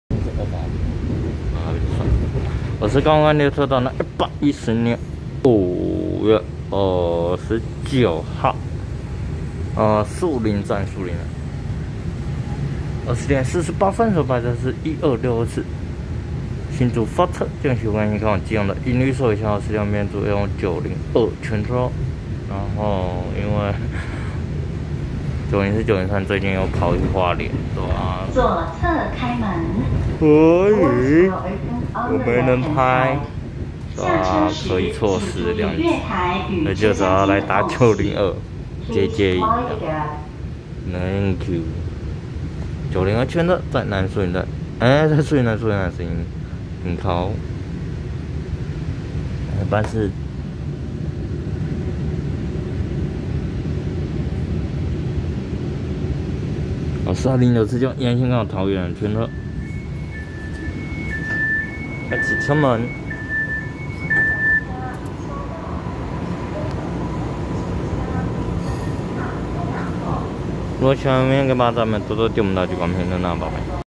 1262次新竹發車經由西部幹線開往基隆的銀綠色微笑號EMU900型十輛一編組EMU902區間車
110/5/29樹林站20:48分拍攝1262次新竹發車經由西部幹線開往基隆的銀綠色微笑號EMU900型十輛一編組EMU902區間車